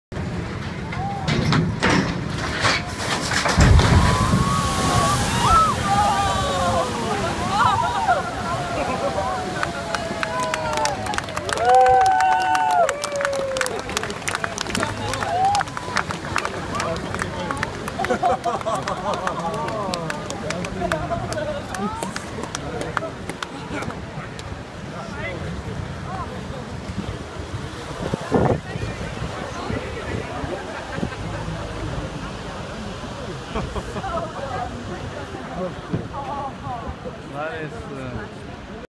Deep Helder Vessel Launch
The Deep Helder Vessel was Launched at De Hoop Shipyard in Foxhol!